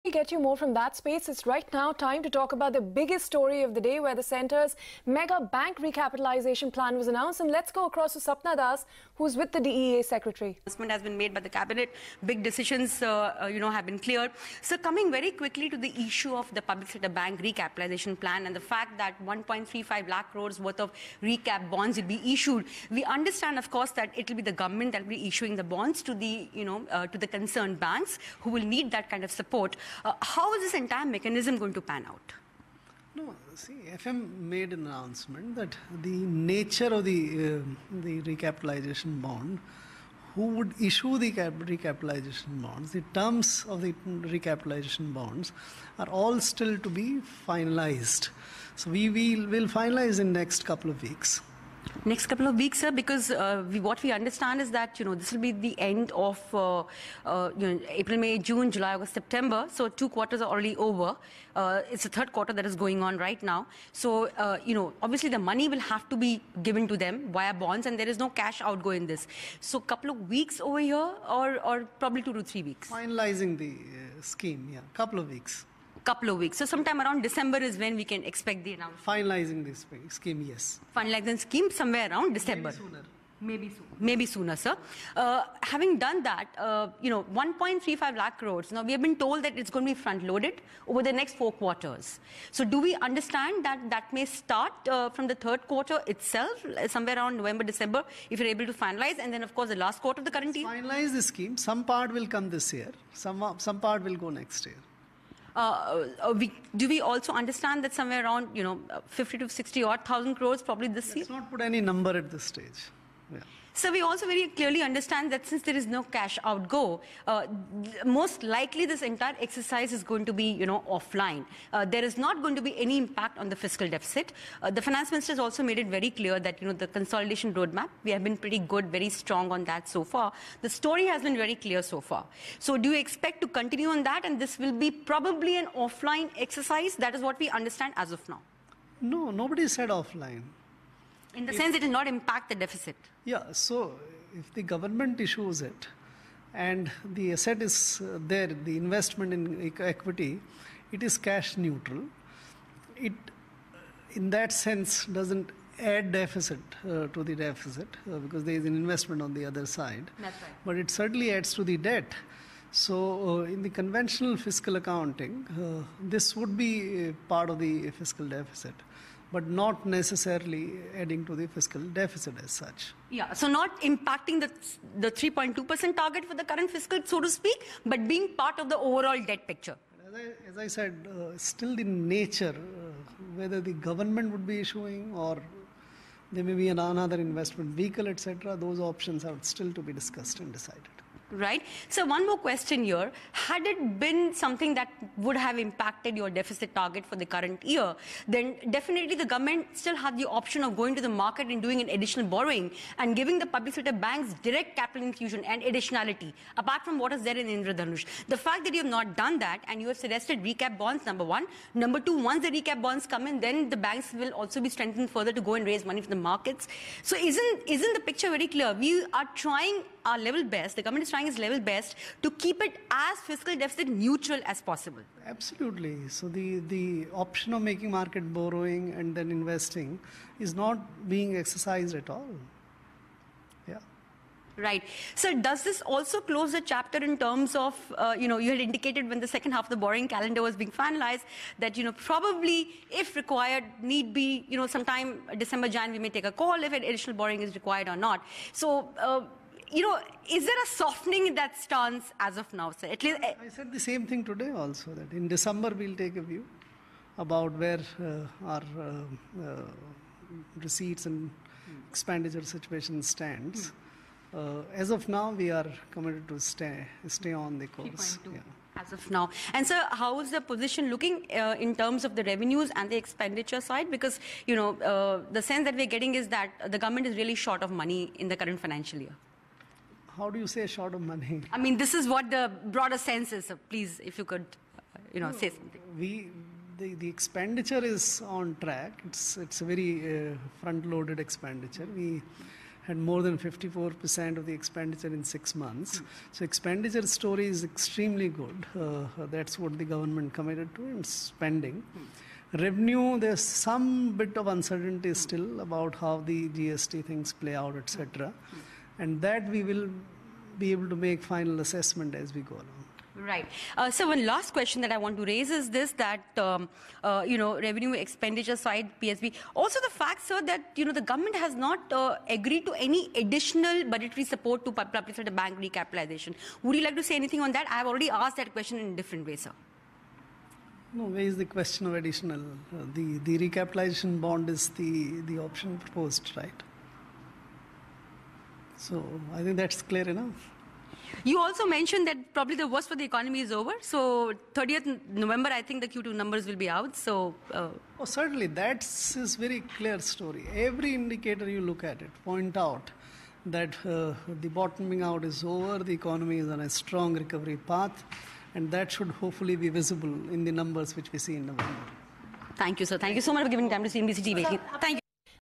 Centre's Recapitalisation Plan
Eco Affairs Secretary SC Garg speaks to CNBC-TV18 about the mega recapitalisation plan, says the bank recapitalisation bonds are not likely to impact the fiscal deficit target